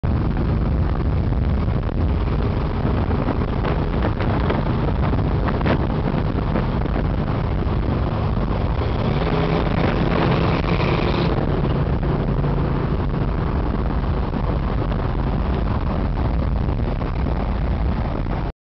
1964 Pontiac Catalina Sport Coupe
A Quick Stomp on the Gas with my GTO Powered 1964 Pontiac Catalina Sport Coupe.